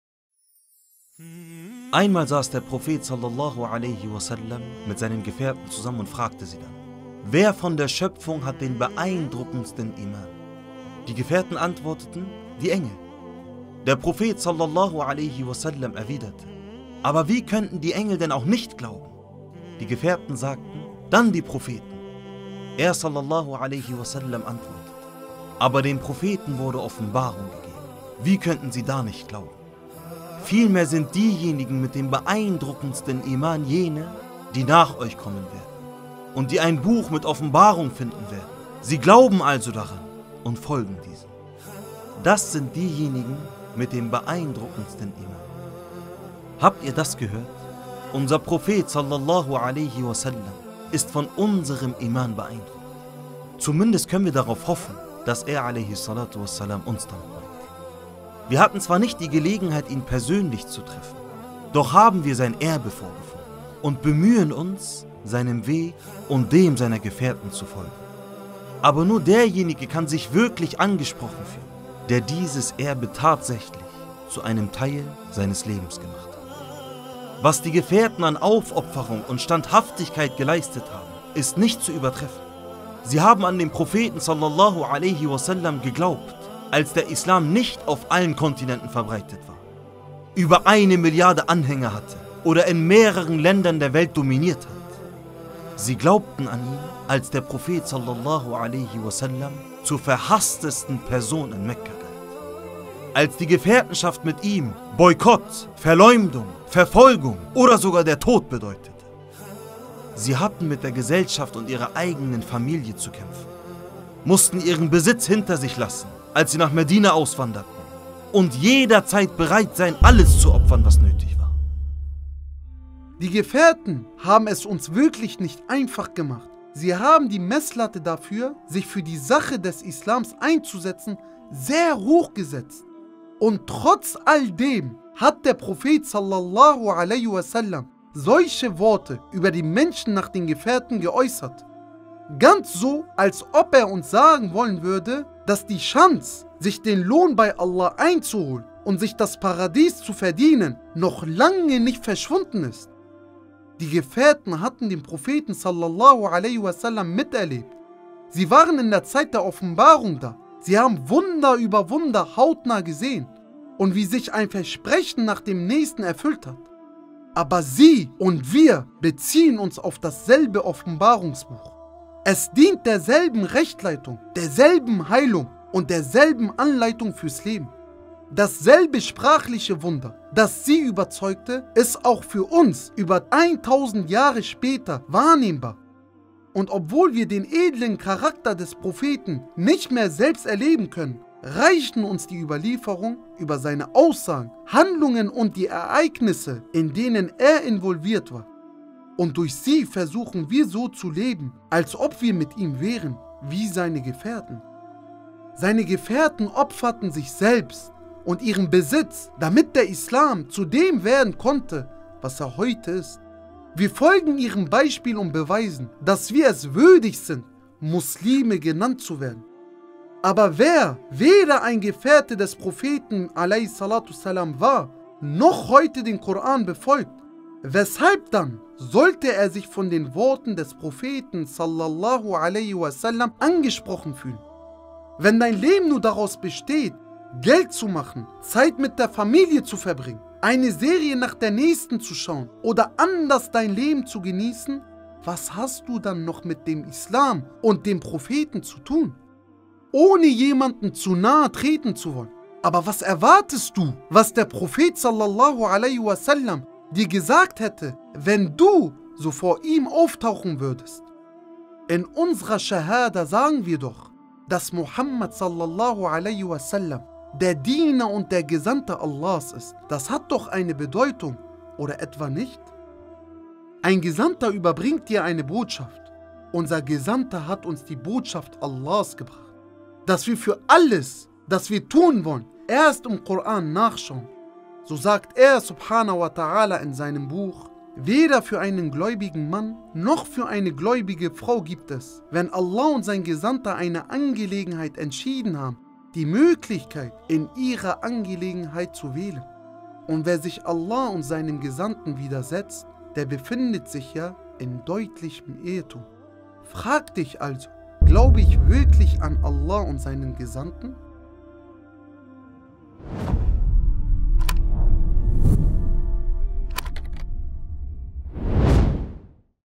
Der Prophet (sas) erwähnte uns! ᴴᴰ ┇ Worte zum Nachdenken ┇ BDI
Einmal saß der Prophet (sas) mit seinen Gefährten zusammen und fragte sie dann: „Wer von der Schöpfung hat den beeindruckendsten Iman (Überzeugung)?